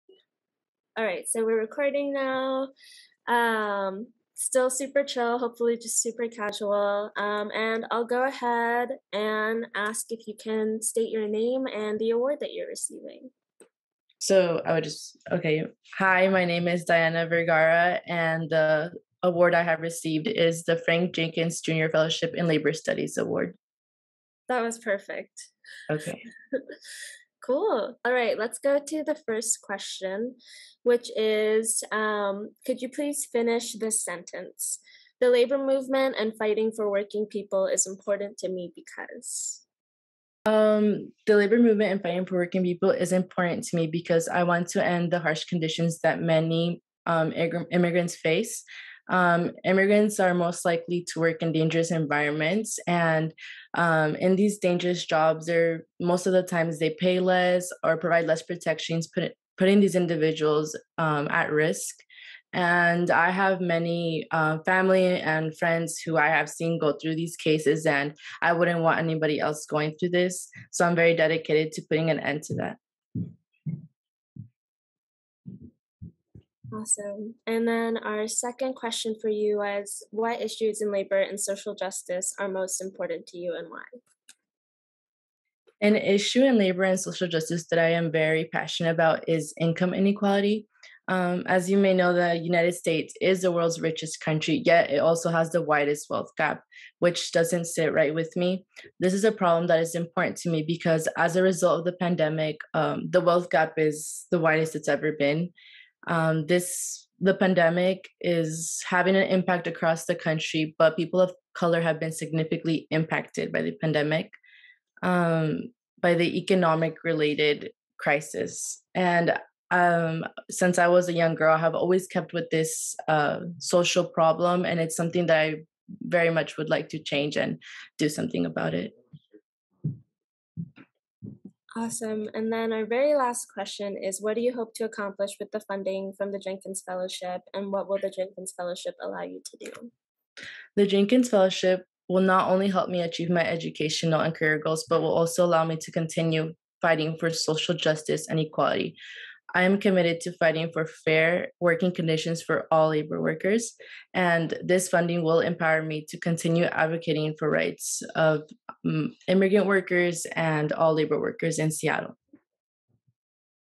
Diana Vergara Full Interview (2021-2022 Scholarship Recipient)
Diana Vergara, University of Washington

An incoming undergraduate, Vergara aspires to become a political leader and a fearless advocate of Black, Latinx, and immigrant populations in Washington state. Vergara started participating in labor, immigrant justice, and feminist movements at a young age. Currently, she is a part of the Latino/a Educational Achievement Project (LEAP), a statewide effort to improve the academic achievement of Latinx students in Washington. Most recently, as a core member of Centro Latino, Vergara has been advocating for more ethnic art in the state capitol and the ratification of House Bill 1372 which requires the removal of statues upholding white supremacy. At UW, Vergara will continue her activism in local politics, get involved on campus to make UW more equitable, and ultimately pursue an education that allows her to grow as a champion of social justice.

Award: Frank Jenkins Jr. Fellowship in Labor Studies

This new award honors the legacy of Frank Jenkins Jr, a lifelong civil rights and union rights activist and one of the first African Americans to hold a leadership role within Seattle’s International Longshore and Warehouse Union, Local 19.